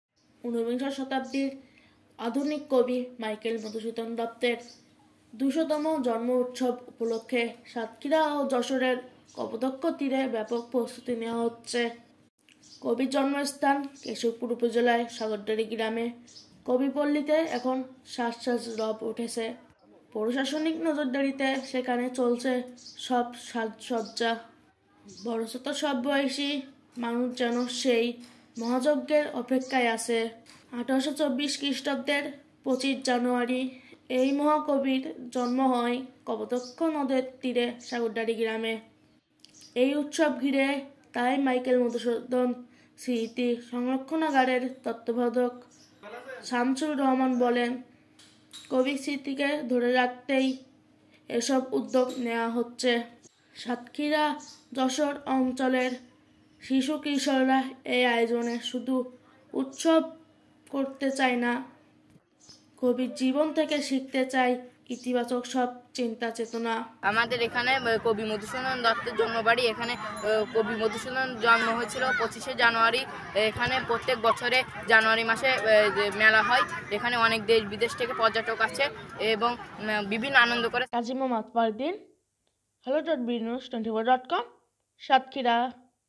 মধুসূদনের জন্মোৎসব পালনে প্রস্তুতি
মাইকেল মধুসূদন দত্তের জন্মোৎসব পালনে নানা প্রস্তুতি নেওয়া হয়েছে।